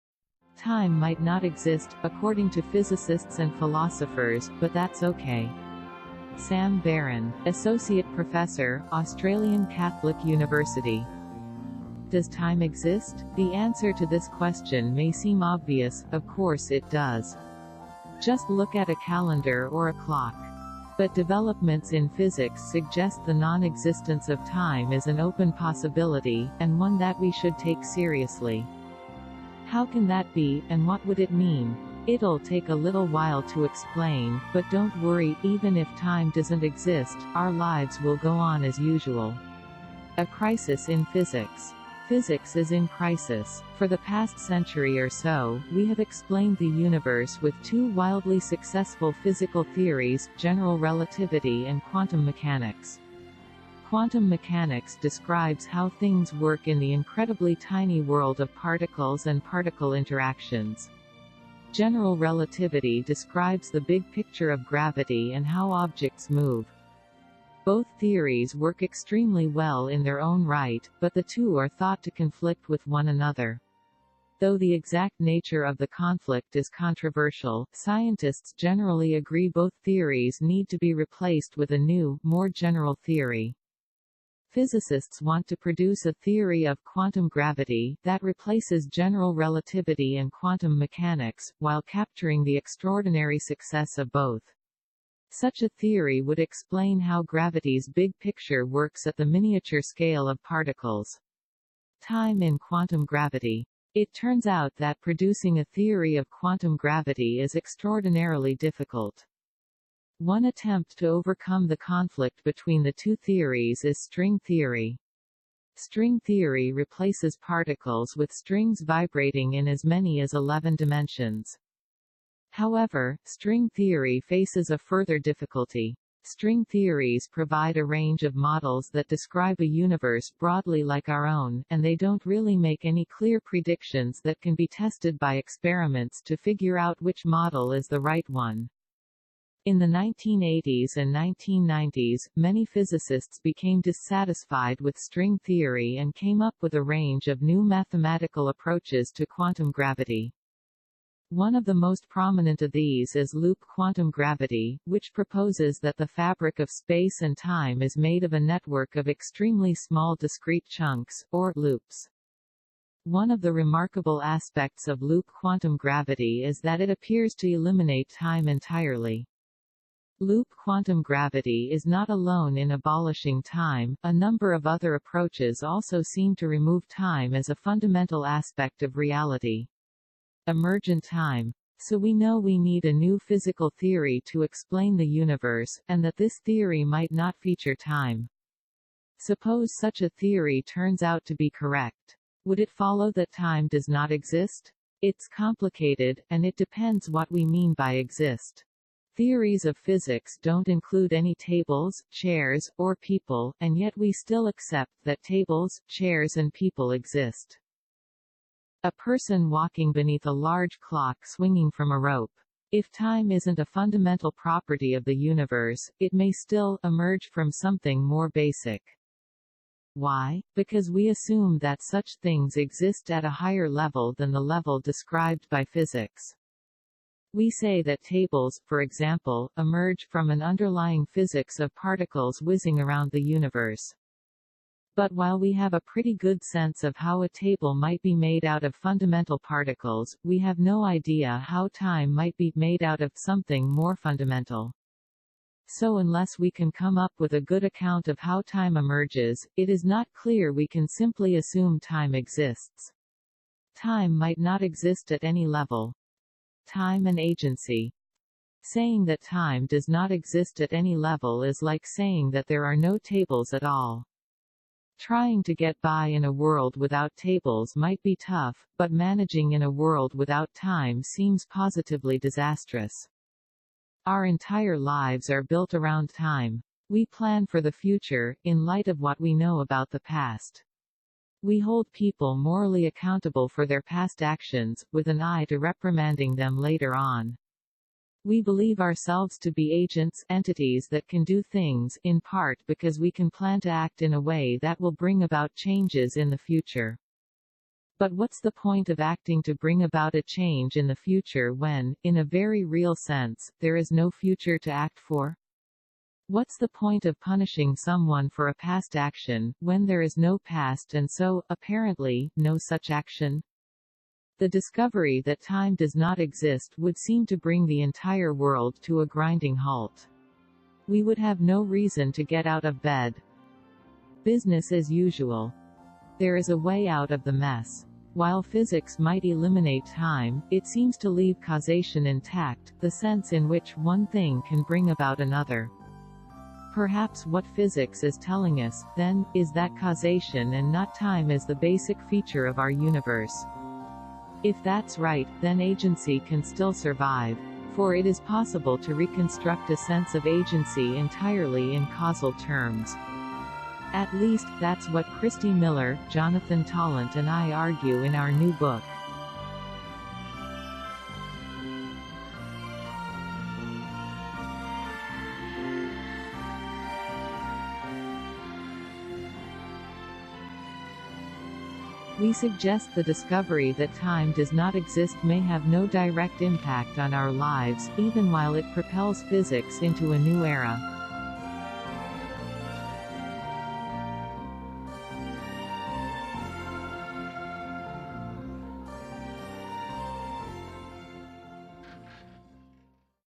Time might not exist, according to physicists and philosophers – but that’s okay
"Does time exist? The answer to this question may seem obvious: of course it does! Just look at a calendar or a clock."
Author: Sam Baron, Associate professor, Australian Catholic University